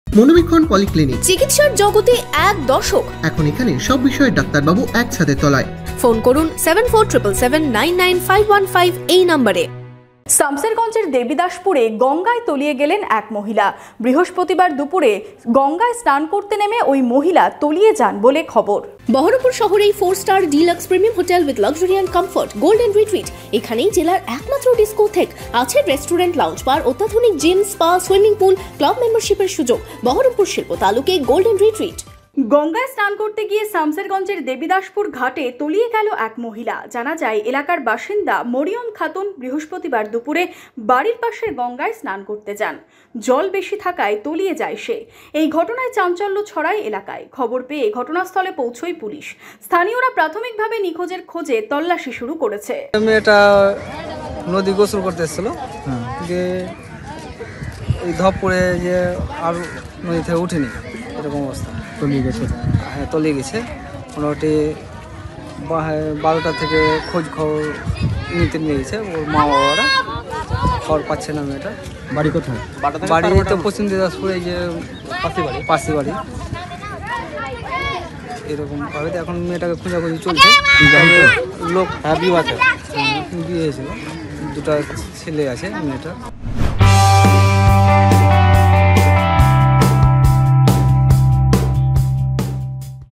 Samsherganj Incident: সামশেরগঞ্জে স্নান করতে গিয়ে গঙ্গায় তলিয়ে গেল গৃহবধূ
A woman drowned while bathing in the Ganga at the Debidaspur Ghat in Samsherganj. According to reports, Mariam Khatun, a resident of the area, went to bathe in the river near her house on Thursday afternoon. Due to the high water levels, she was swept away. This incident caused a stir in the locality. Upon receiving the news, the police arrived at the scene, and locals have started an initial search to find the missing woman.

Madhyabanga News